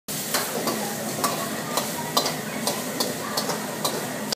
The Chinese Cook at Ocean's Wok Paradise Sittard
This Chinese cook made the best wok dinner for me.